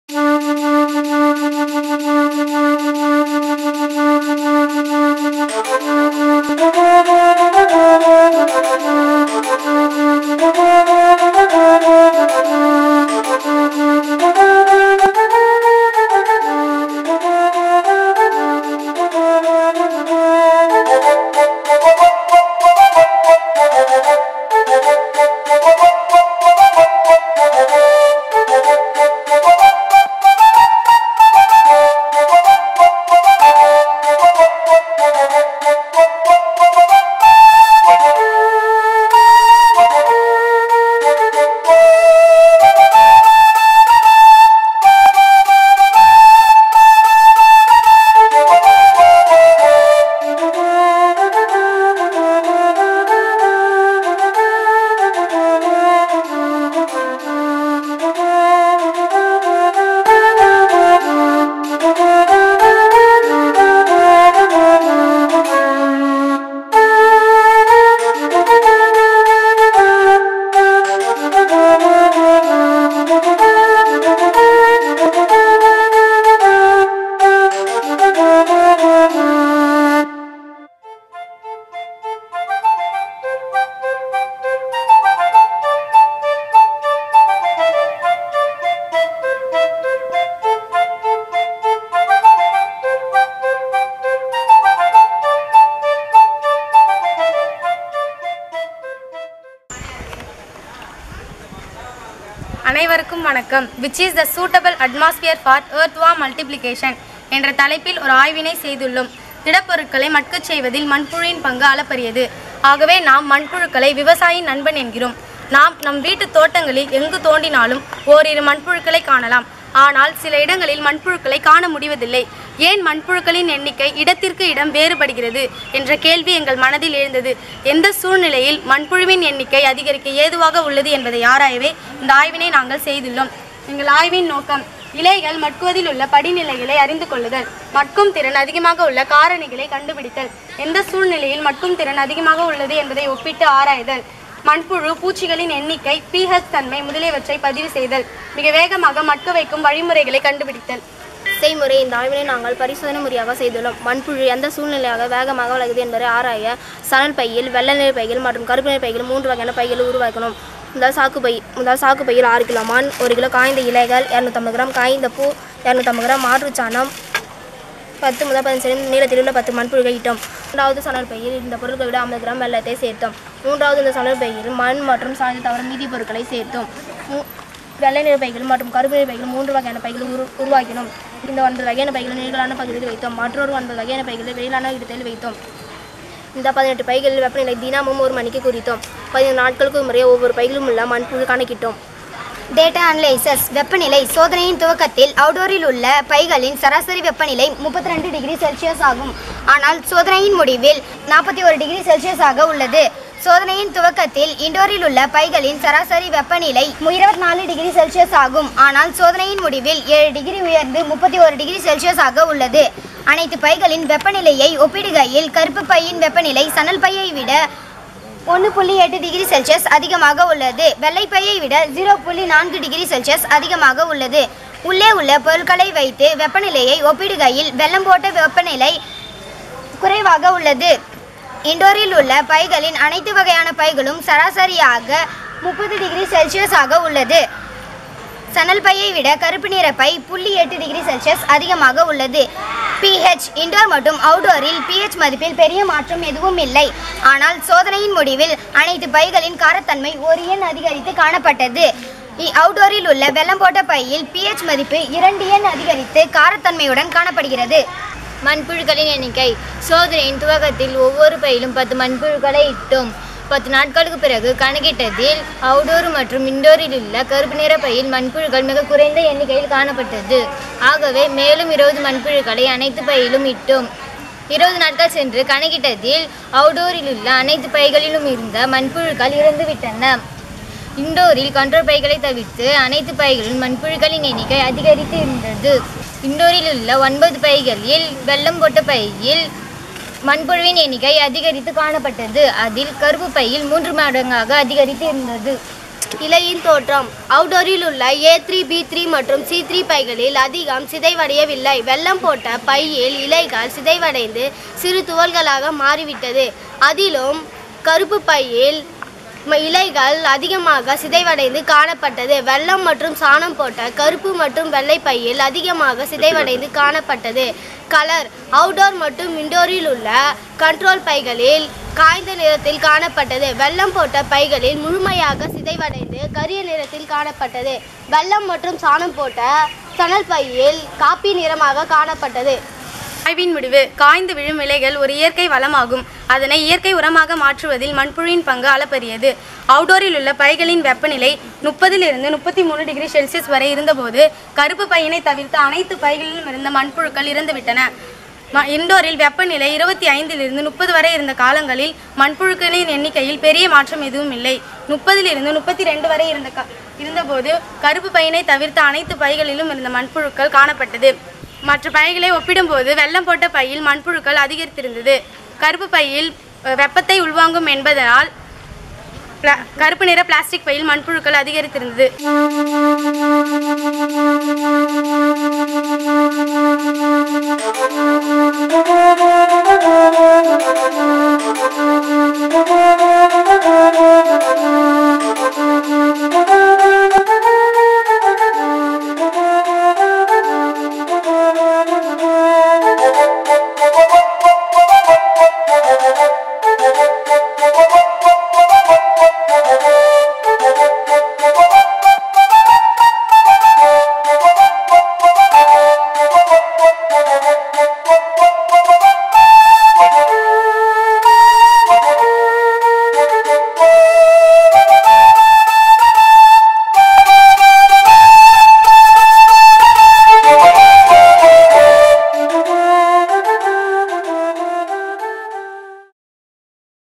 No.10: faites de la science-Make Science Competitions 2017
Part 10of 12 videos of Make Science Competitions 2017 - Edition XI in collaboration with University of Paris, Sud 11, France. Produced by Pondicherry Science Forum.